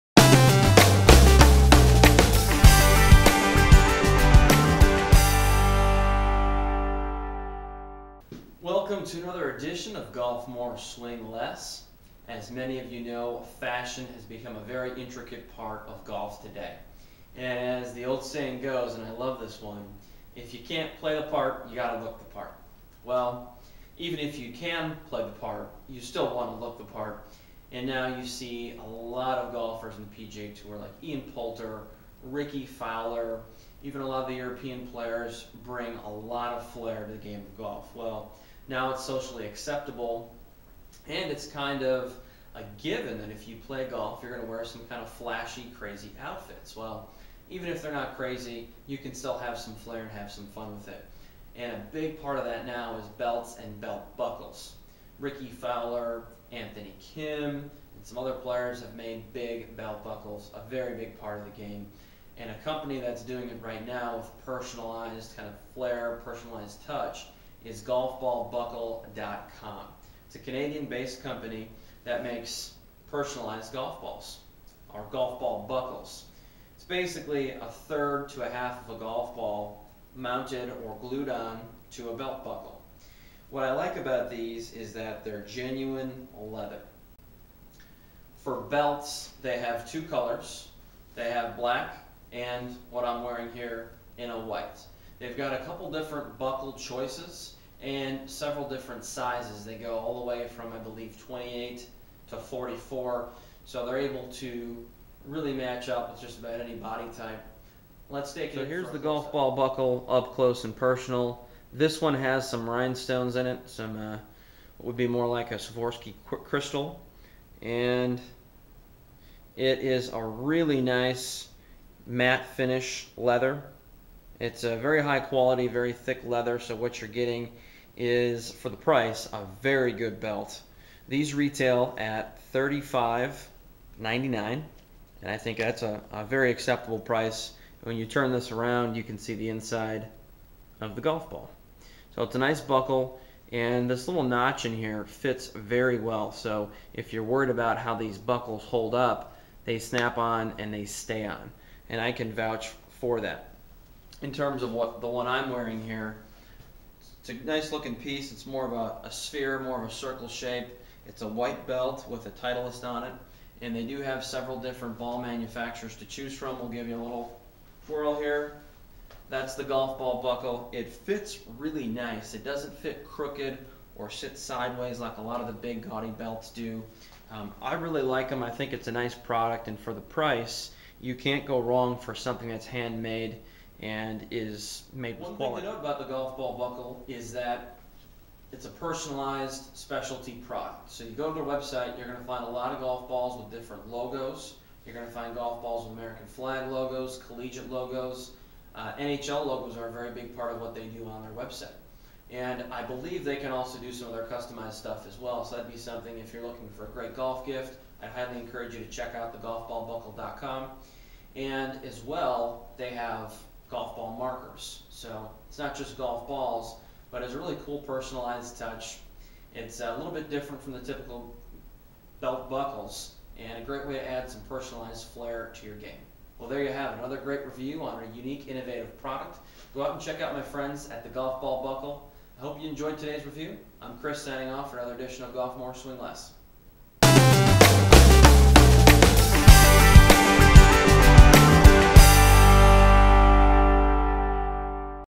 Golf Ball Buckle - Custom Belt Buckle Product Review
Golf More Swing Less Blog - Reviews the Golf Ball Buckle. These belts and belt buckles are custom and personalized with a variety of styles and logo options.